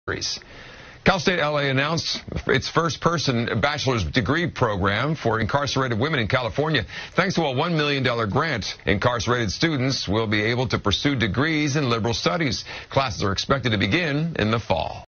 Cal State LA to launch first bachelor's degree program for incarcerated women in CA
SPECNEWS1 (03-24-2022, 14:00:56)